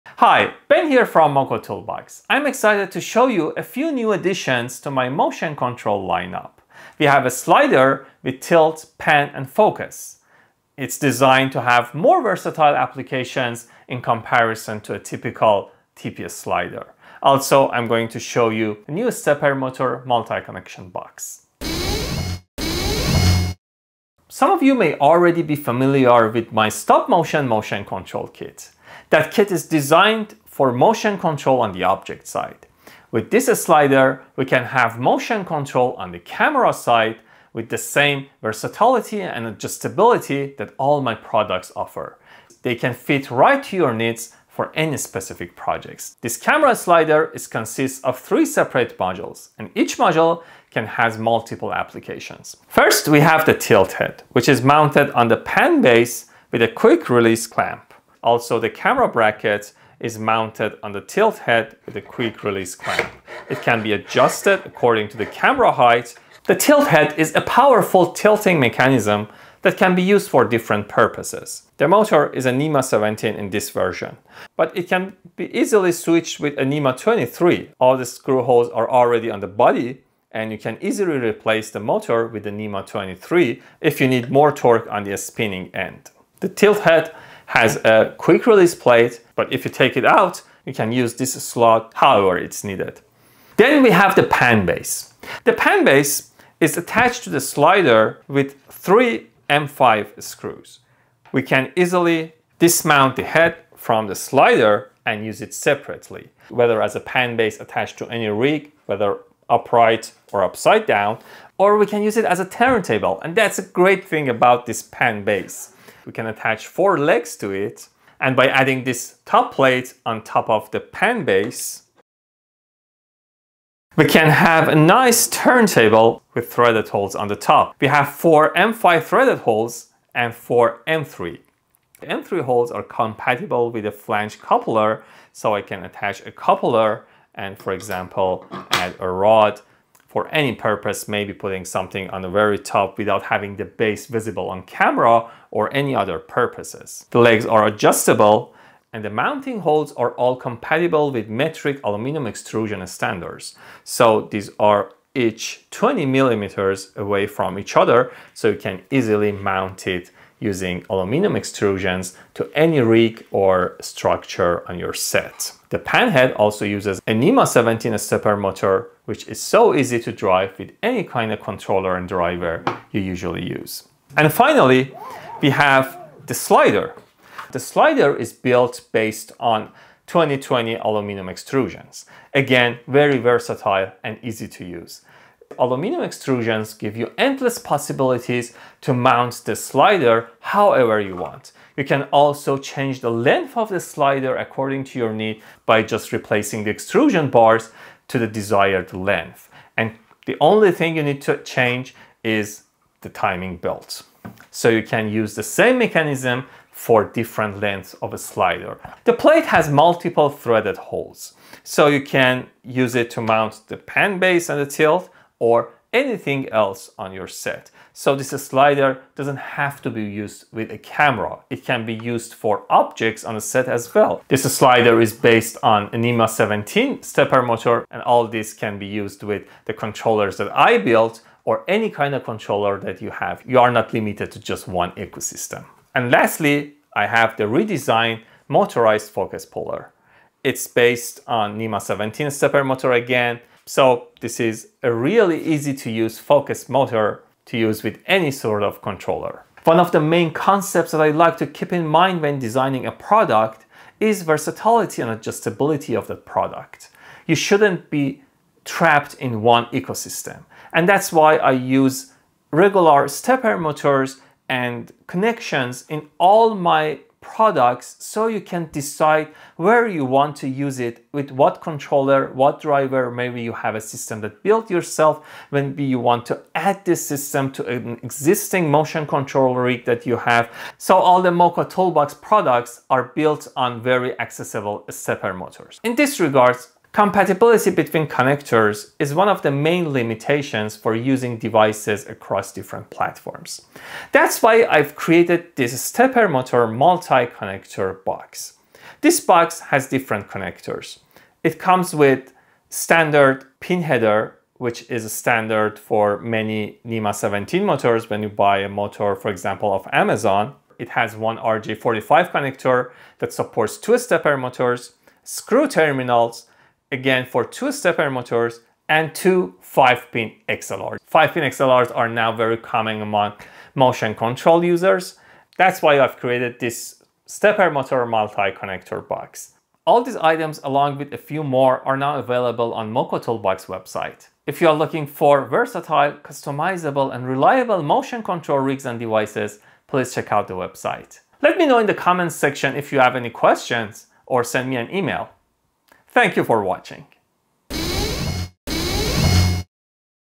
NEW! Modular Motorized Camera Slider from Moco Toolbox IS ALSO a Turntable!
The Moco Toolbox Modular Motorized Camera Slider is finally here! This isn't just a typical TP Slider ; it's a complete, modular motion control system designed for ultimate versatility and adjustability.

This kit consists of three separate modules: the tilt head, the pan base, and the slider, and each module has multiple applications.

The biggest breakthrough is the Pan Base, which can be easily dismounted from the slider and converted into a standalone MOTORIZED TURNTABLE. It's perfect for product videography, time-lapses, and smooth 360° object shots.

All Moco Toolbox products are built on highly accessible Nema 17 stepper motors, meaning you are not trapped in a single ecosystem and can use the components with any controller or driver you usually use.

0:00 - Introduction to the Tilt, Pan, and Focus Slider
0:46 - The Three Separate Modules
0:52 - The Powerful Tilt Head (and Nema 23 upgrade option)
1:33 - The Pan Base and Turntable Conversion
2:57 - The Versatile Aluminum Extrusion Slider
3:59 - The Redesigned Motorized Focus Puller
4:53 - The Stepper Motor Multi-Connection Box (RJ45, XLR, Screw Terminal compatibility)
5:34 - Final Thoughts & Where to Buy

*Follow Moco Toolbox for the latest updates:*